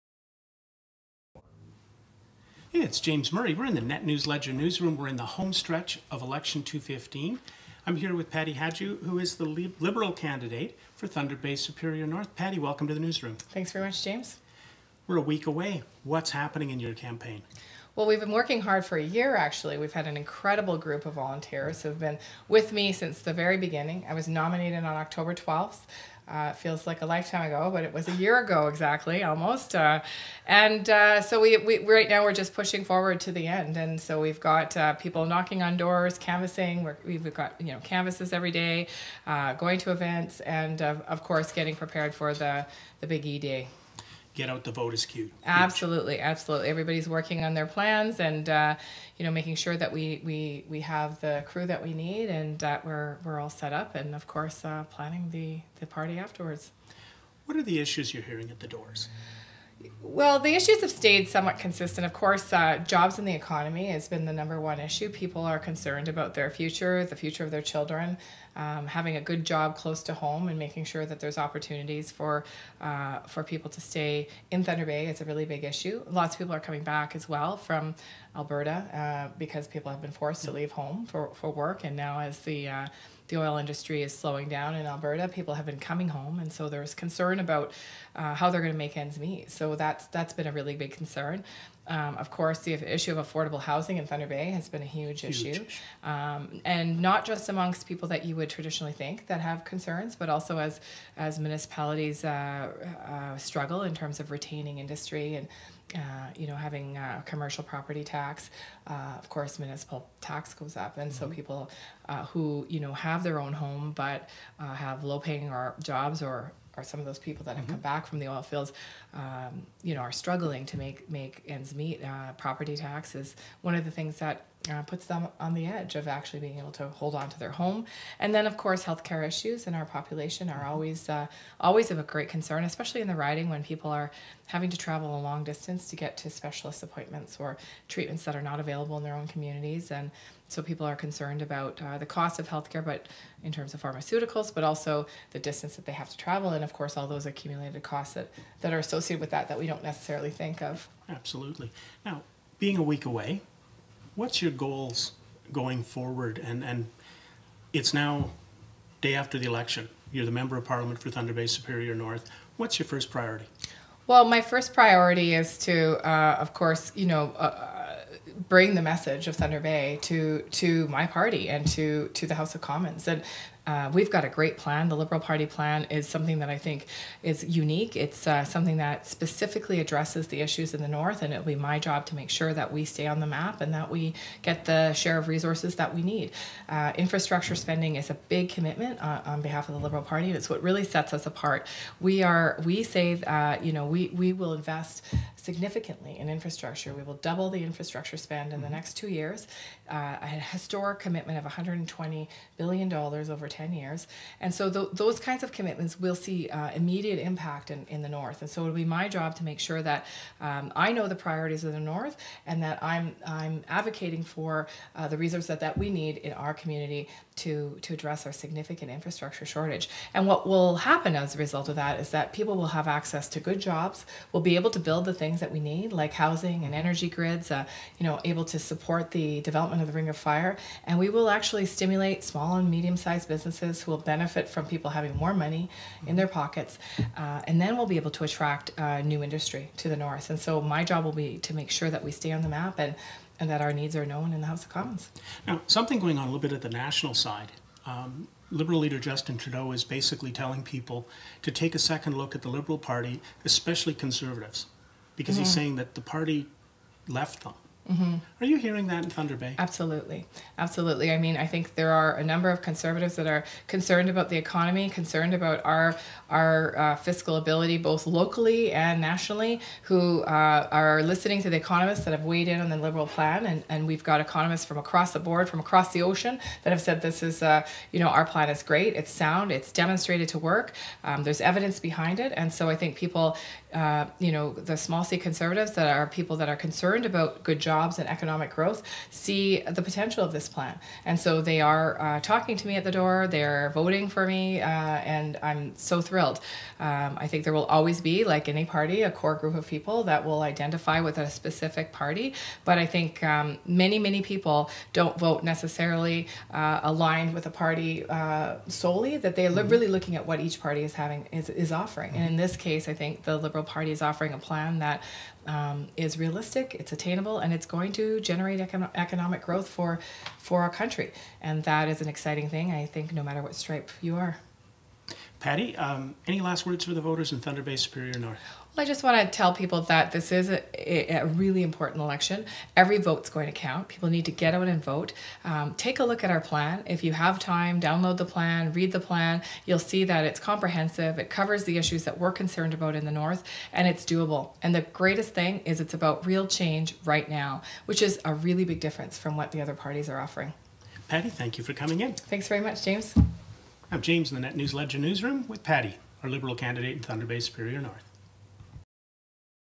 Patti Hajdu Liberal Candidate TBSN October 13 2015
Liberal candidate Patti Hajdu is running in Thunder Bay Superior North. Patti shares the efforts going into the last week of the campaign and how things are going.